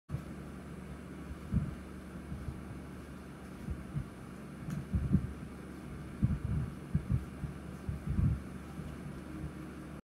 Bad Raccoon Eats My Shoe
Young raccoon loves to steal my shoes. She stole this one and dragged it onto the bed. Raccoons tend to be very playful when raised in captivity and enjoy playing with a variety of toys. Certain toys excite their natural hunting instincts and others are simply fun for them, like my shoe.